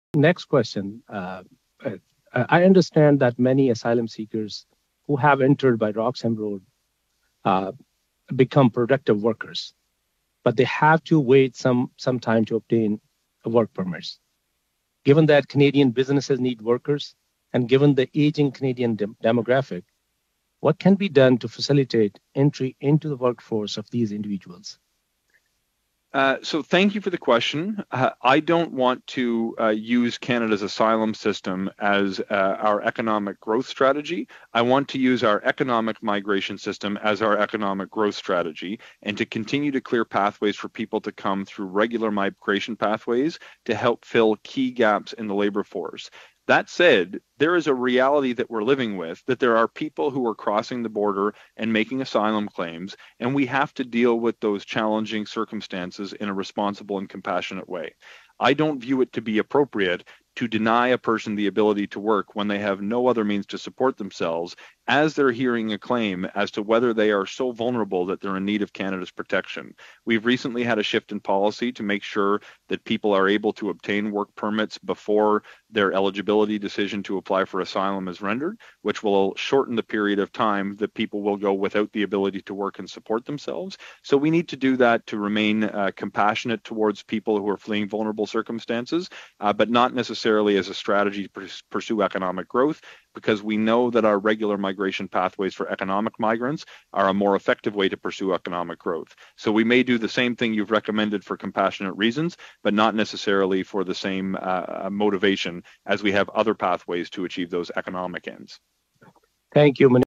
I asked to IRCC About facilitate the entry of these asylum seekers into workforce 2 help businesses
Shafqat Ali: Today, during the Standing Committee on Citizenship and Immigration study of conditions faced by asylum-seekers entering Canada irregularly at Roxham Road, I asked the Honourable Sean Fraser , Minister of Immigration, Refugees and Citizenship, what could be done to facilitate the entry of these asylum seekers into the workforce to help businesses who are short of workers.